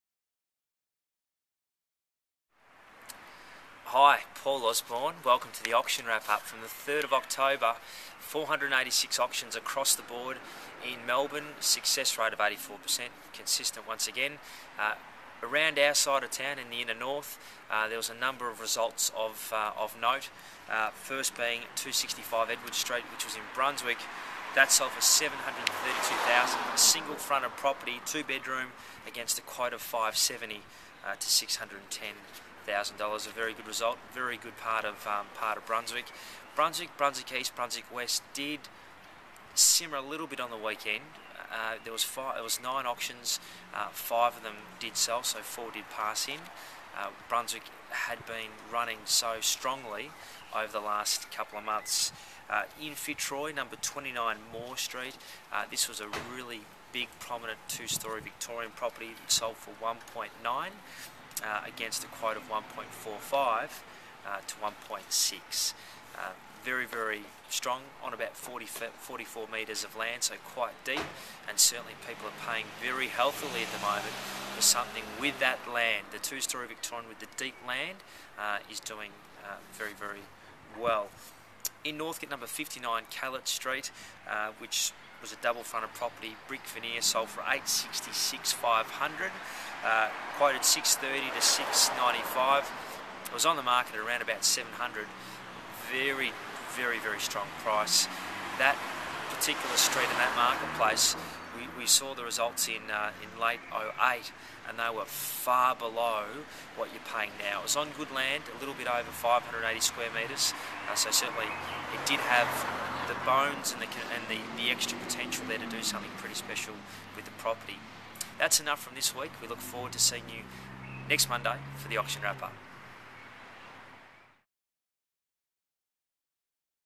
Auction Wrap for October 3rd 2009
Paul Osborne discusses the results from the auction market this weekend in the inner north.